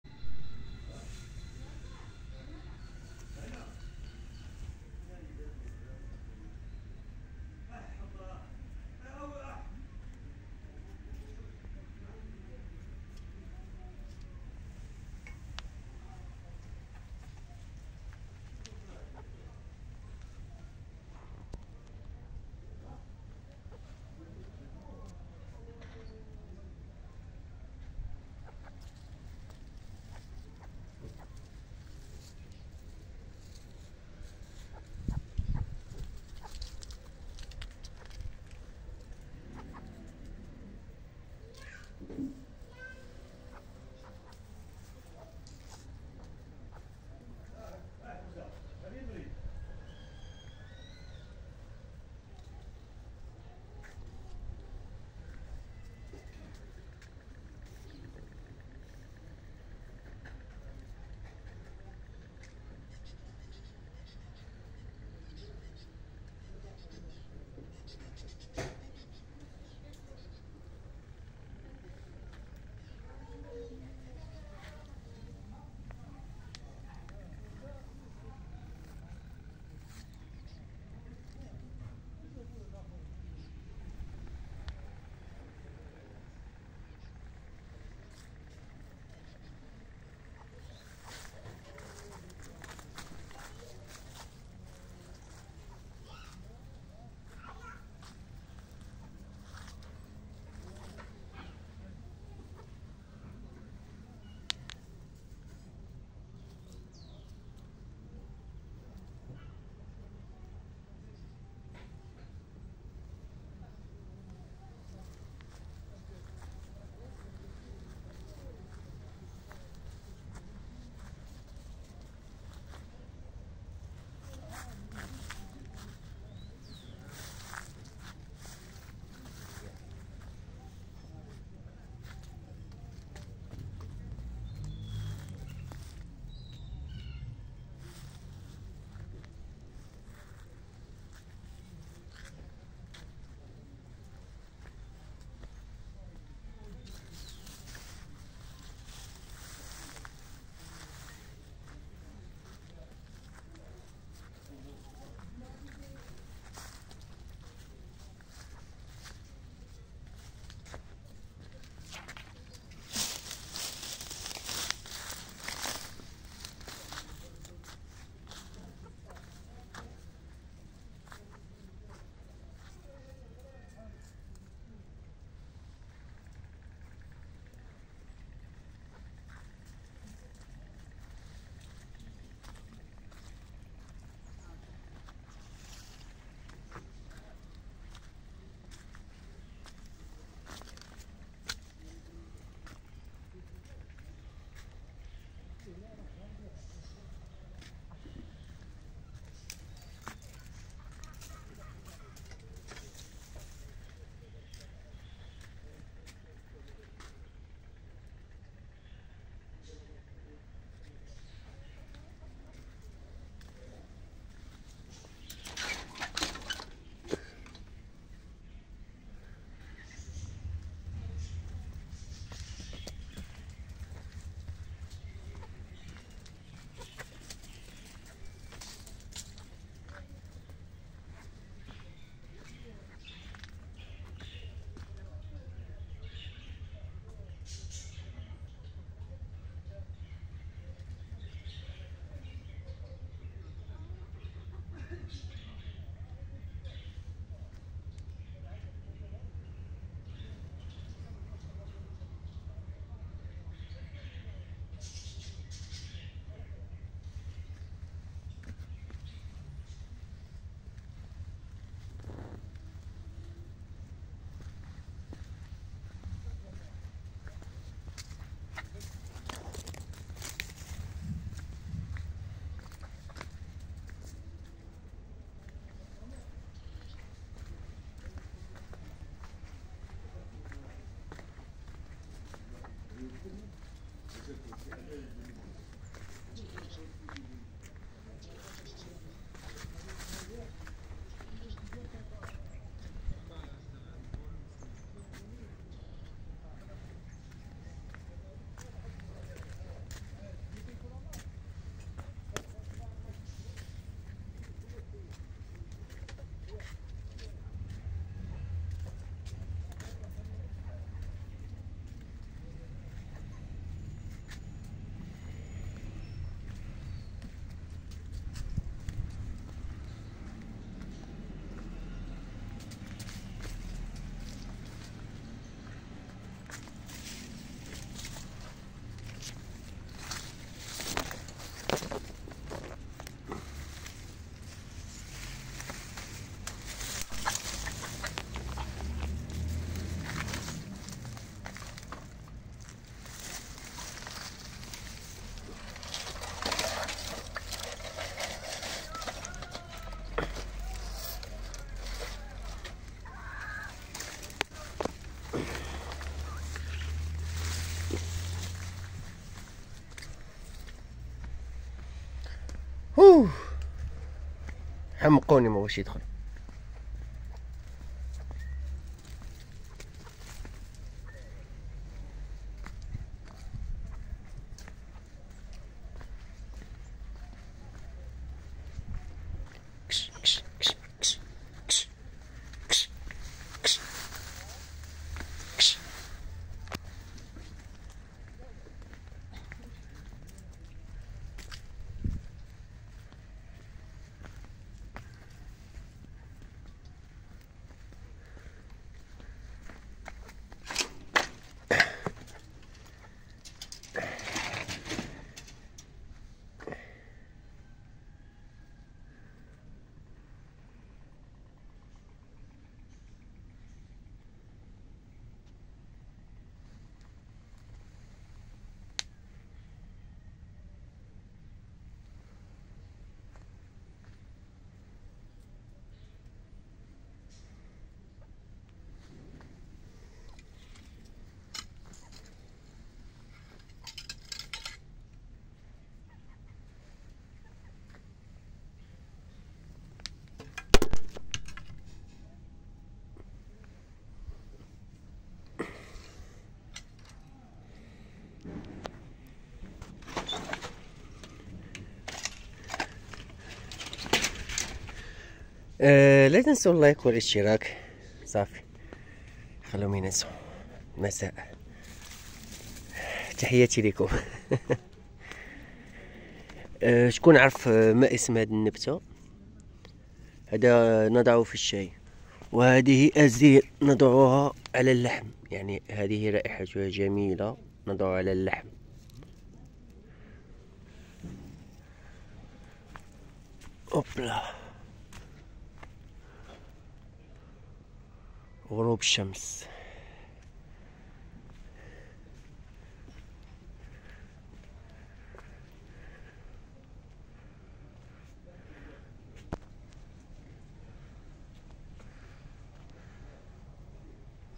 نصيحة مني ليكم خاصك تربي دجاج فهاذ الوقت أزمة جاية فلحوم بيضاء
🚩  حتى توصلك الفيديوهات الجديده على طول و بأسرع وقت